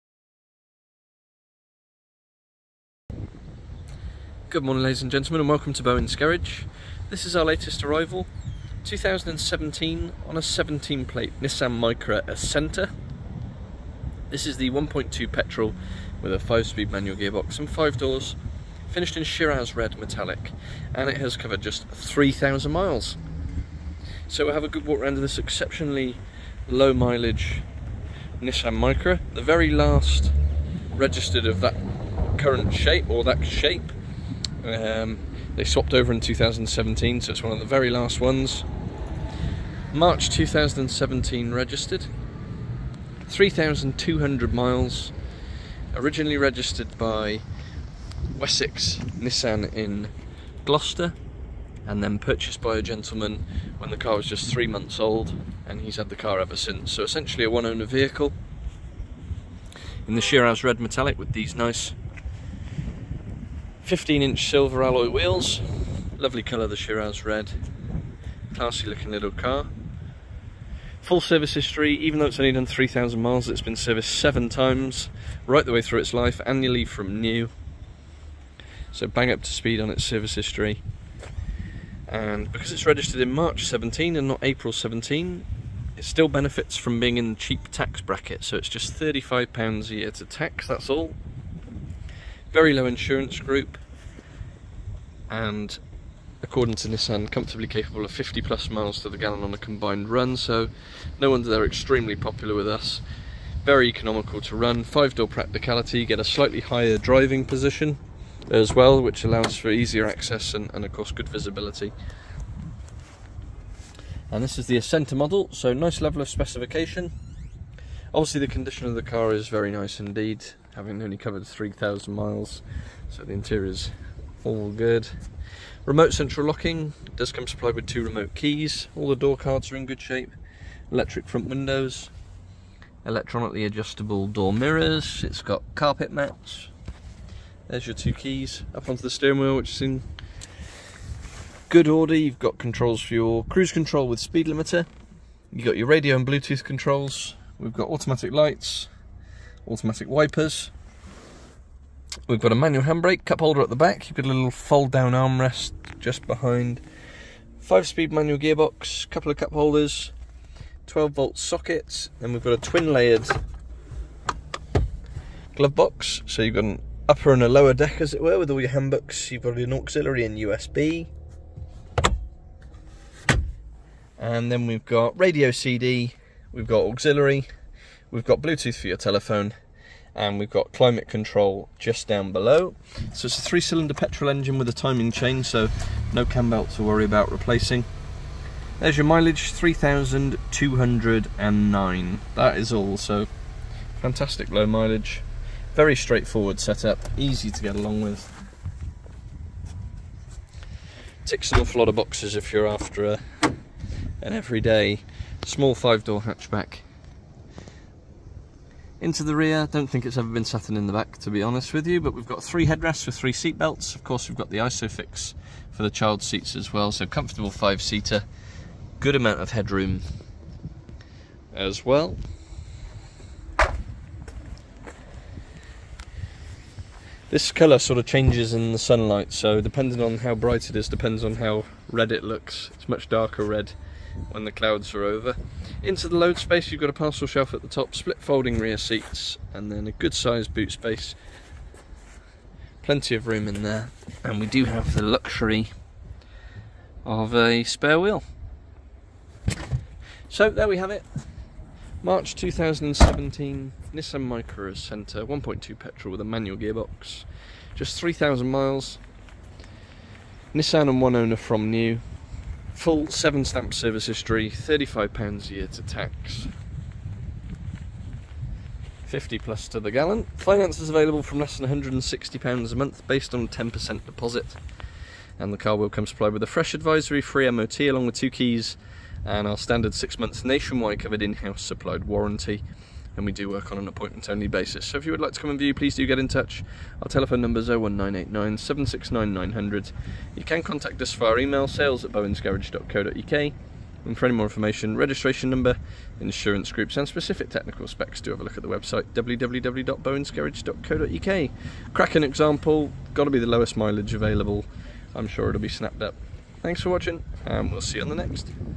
2017 (17) Nissan Micra Acenta 1.2 5Dr in Shiraz Red. 3k Miles.Demo +1 Owner.7 Services.£35 Tax.£8000
Reg Number: WR17TKA

Just arrived this lovely little Micra Acenta with just 3k miles on the clock, yes just 3k!.

Owned by the current gentleman since 2 months old in 2017 after being registered with Nissan it has been really well maintained despite the super low mileage with 7 recorded services, 6 of which carried out at Nissan.

Looks & drives very well indeed & comes nicely appointed with the likes of Climate Control, Cruise Control with Speed Limiter, Alloy Wheels, Auto Lights & Wipers, Electric Windows & Mirrors, AUX/USB Inputs, Bluetooth, the list goes on.

It is also just £35 a year to tax & capable of 50+ MPG so a really economical, reliable 5 door hatch.

It will come supplied with a fresh advisory free MOT, Oil Service & 2 Keys along with our standard 6 month, nationwide covered, in house supplied warranty.

A good value, exceptionally low mileage little car.

Viewings available at either our Ross-On-Wye Branch HR9 7BW or our central Hereford Branch HR1 1EN.